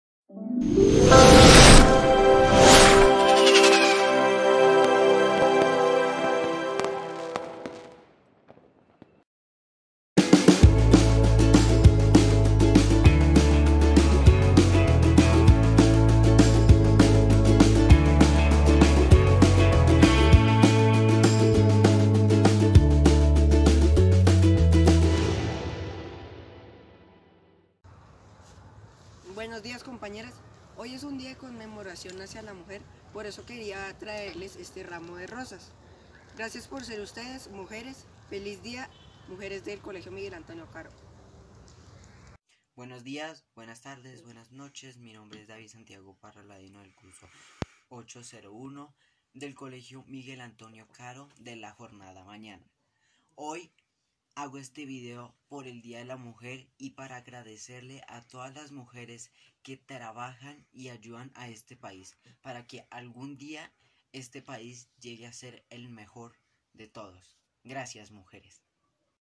DIA INTERNACIONALDE LA MUJER
Video elaborado por dos estudiantes del curso 801 JM del Colegio Miguel Antonio Caro (IED) de la jornada mañana, un reconocimiento para sus compañeras de curso y de las mujeres de la comunidad MAC (Miguel Antonio Caro)